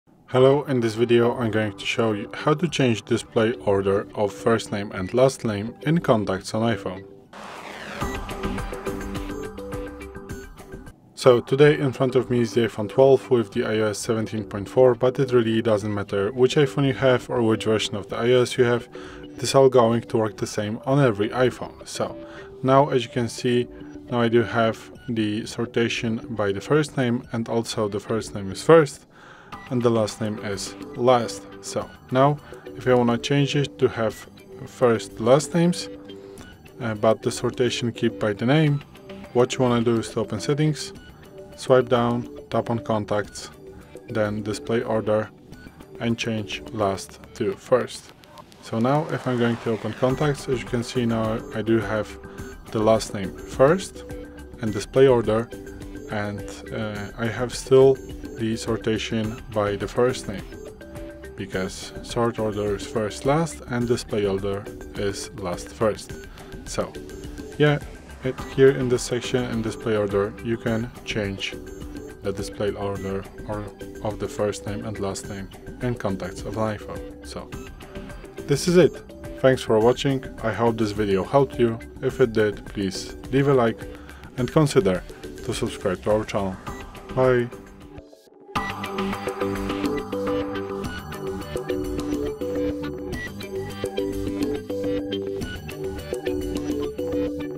How to Change Name Display Order in iPhone Contacts
Find out more about your device:
Learn how to customize the display order of names and surnames in your iPhone's Contacts app. This tutorial provides clear instructions on adjusting settings to display names in either "First, Last" or "Last, First" format, according to your preference.

Can I switch names to display last name first on iPhone?
Where are name display settings in iPhone Contacts?
Does changing name order affect other synced apps?
Can I revert name display changes in iPhone Contacts?